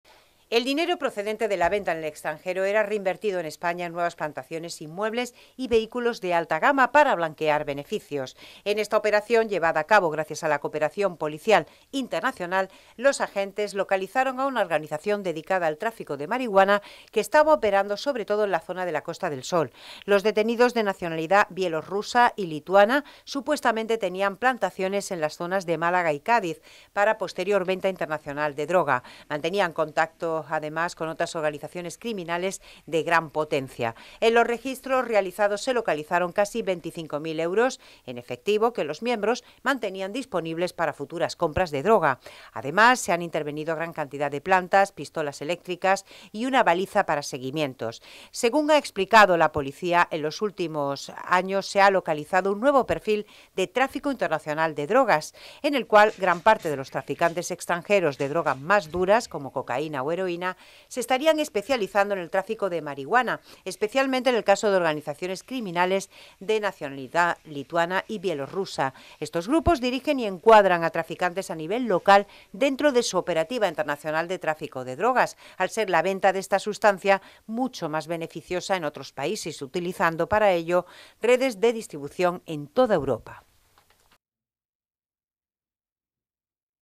Cinco detenidos de una organización con plantaciones de marihuana en Cádiz y Málaga
Agentes de la Policía Nacional, en una operación conjunta con la Policía de Bielorrusia e Interpol, han desarticulado una organización internacional que supuestamente traficaba con grandes cantidades de marihuana que enviaba al resto de Europa. En este operativo han sido detenidas cinco personas que presuntamente tenían plantaciones en Málaga y Cádiz.